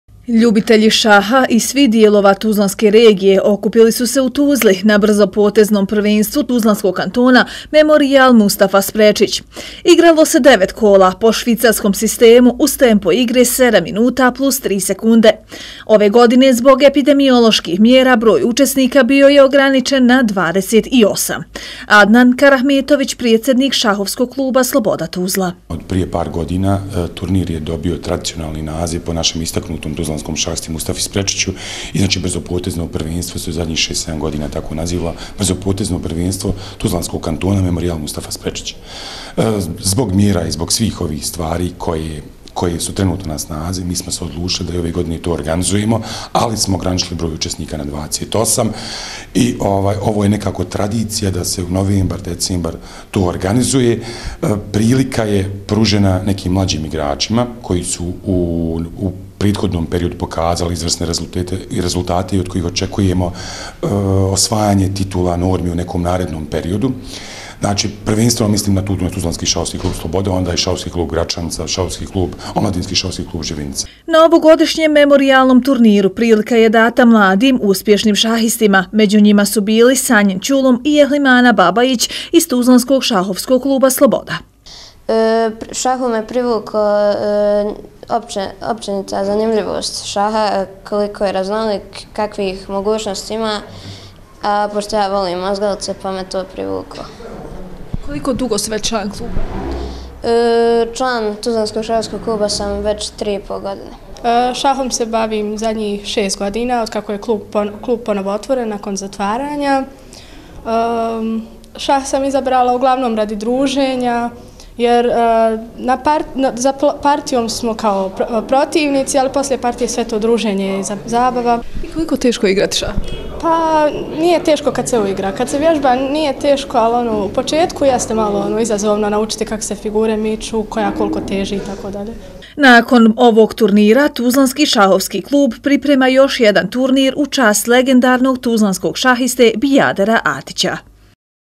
ŠK "Sloboda" organizovala šahovski “Memorijal Mustafa Sprečić”
Ljubitelji  šaha iz svih dijelova tuzlanske regije okupili su se u Tuzli, na         brzopoteznom prvenstvu Tuzlanskog kantona “Memorijal Mustafa Sprečić". Igralo se devet kola po švicarskom sistemu, uz tempo igre 7 minuta plus 3 sekunde. Zbog epidemioloških mjera broj učesnika i učesnica je ove godine  bio je ograničen na 28. Ove godine, prilika je data mladim, uspješnim šahistima.